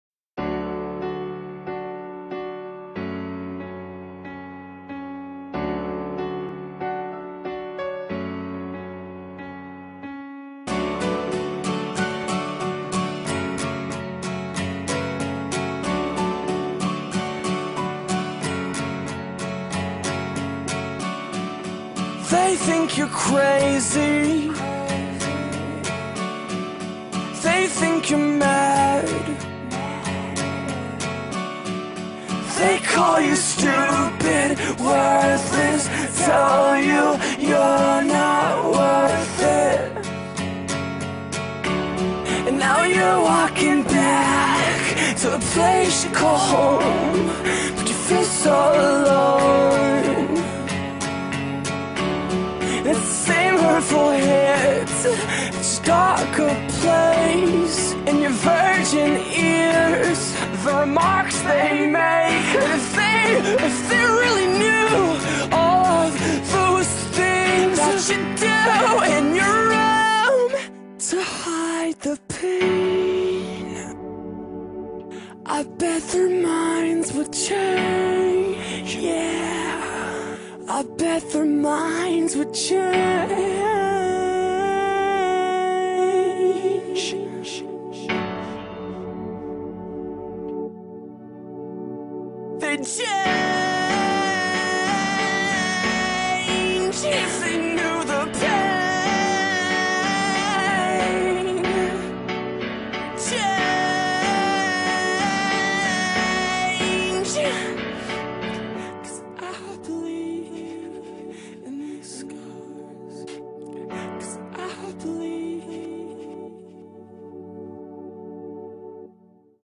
Dunmore CS YSI - Raising awareness about self harm
This video was made by a student in Dunmore Community School as part of a YSI project with the hope of raising awareness about self harm :) Please like our facebook or follow us on Twitter